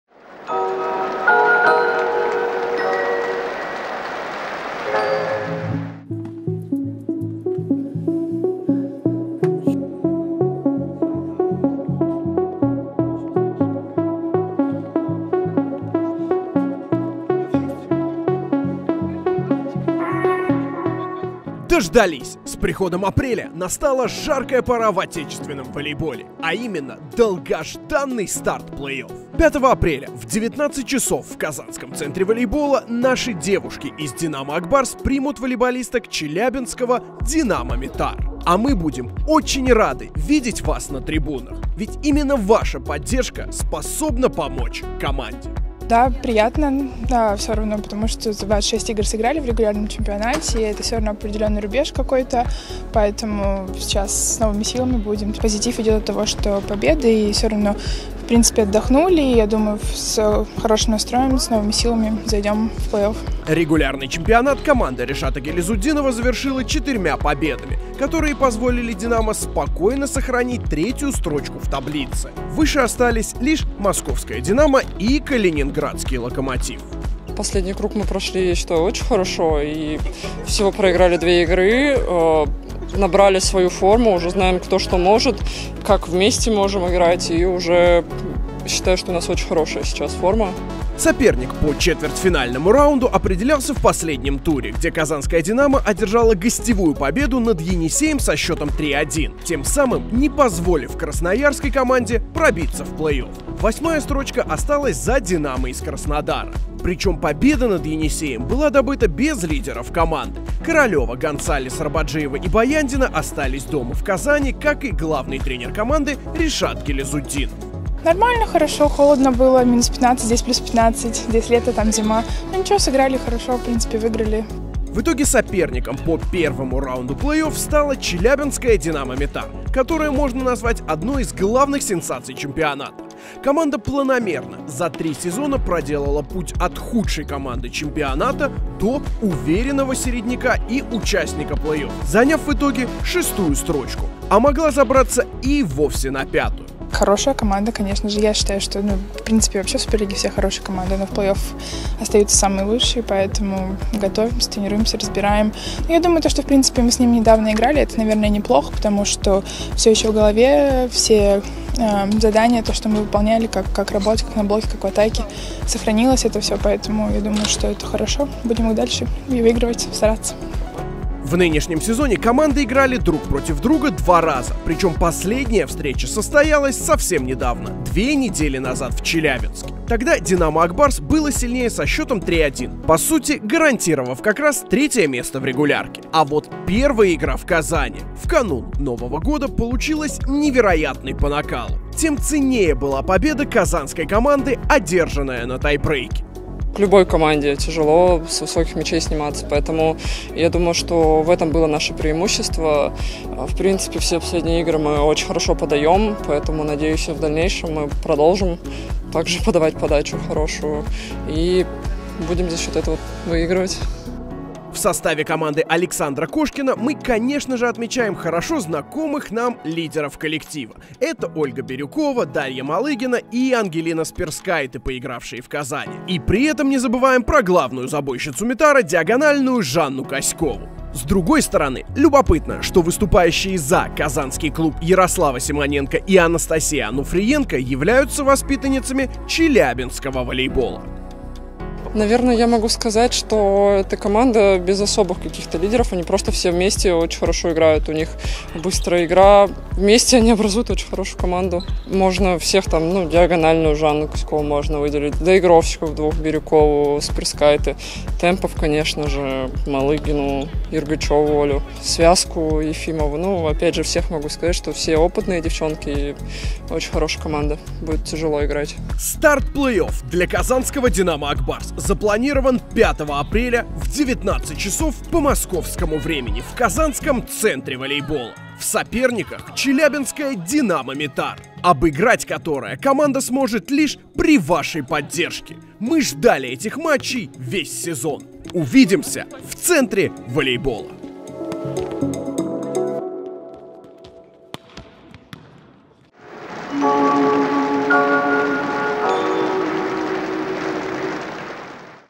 Впереди плей-офф | Динамо-Ак Барс - Динамо-Метар
В четвертьфинале плей-офф PARI Суперлиги «Динамо-Ак Барс» встретится с челябинским клубом «Динамо-Метар».
Комментарии и ожидания игроков от предстоящей встрече в нашем новом видео